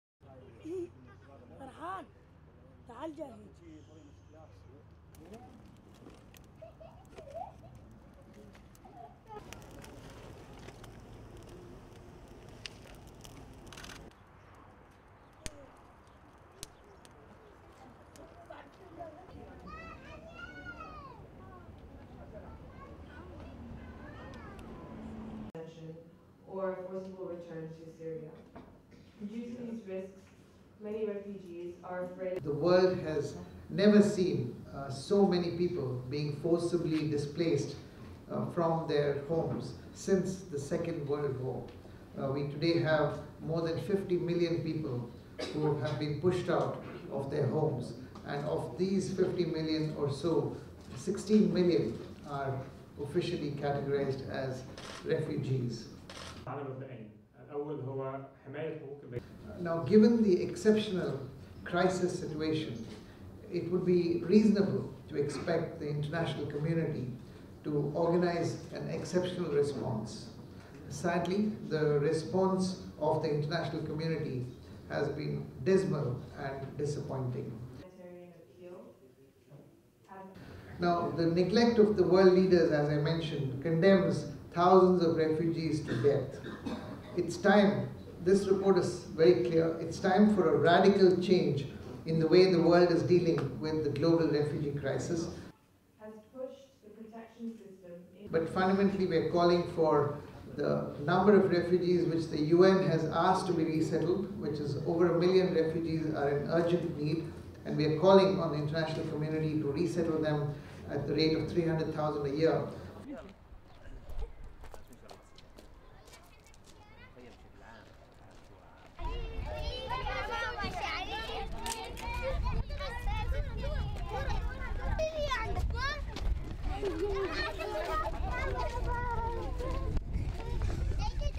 Amnesty calls for radical change in refugee response
(15 Jun 2015) LEAD IN:
Amnesty International is urging world leaders to radically change how they deal with refugees.
The human rights watchdog has published a report today (Monday) saying millions of vulnerable people have been abandoned by the international community.
STORY-LINE:
The children may be smiling, but life here at Zahrani refugee camp is not easy.
These Syrians have fled to South Lebanon, forced from their homeland by the ongoing conflict.
Around 1.2 million Syrian refugees are registered with the UN in Lebanon.
Human rights watchdog Amnesty International says not enough is being done to help them and the victims of other humanitarian crises across the world.
At a news conference in Lebanon, the organisation's Secretary General says the situation is one of historic proportions.
"The world has never seen so many people being forcibly displaced from their homes since the Second World War," says Salil Shetty.
"Today we have more than 50 million people who have been pushed out of their homes and of these 50 million or so, 16 million are officially categorised as refugees," he adds.
From Syria to South Sudan, Myanmar to the Democratic Republic of the Congo, Amnesty says there's a long line of people displaced from their homes.
Its report says world leaders must do more to help them.
"Sadly the response of the international community has been dismal and disappointing," says Shetty.
He calls for "a radical change in the way the world is dealing with the refugee crisis".
Amnesty's report makes a number of recommendations including prioritising saving lives over immigration polices, combating people traffickers and establishing a global refugee fund.
"But fundamentally we are calling for the number of refugees which the U.N. has asked to be resettled, which is over a million refugees are in urgent need," Shetty says.
"We are calling on the international community to resettle them at the rate of 300,000 a year," he adds.
But persuading the world to listen is a difficult task.
The UN's humanitarian appeal for Syrian refugees was only 23 percent funded as of the 3 June 2015 and programmes in other countries are also underfunded.
It's the families living in refugee camps like this who suffer because of the world's inaction.